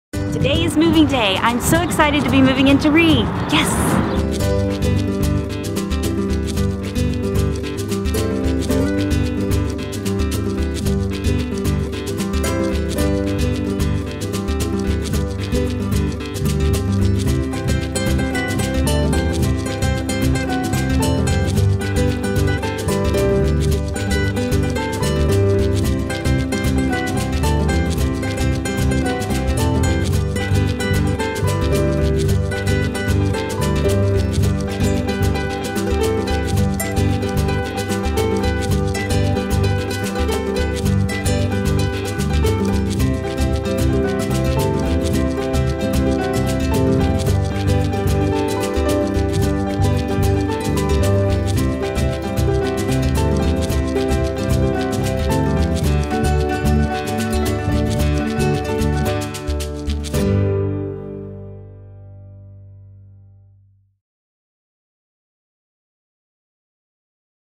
{reDesigner} Nancy Kay moves into Restoration Emporium!
Some fun with GoPros while moving {reDesigner} Nancy Kay into her new space at Restoration Emporium in Kansas City's historic West Bottoms!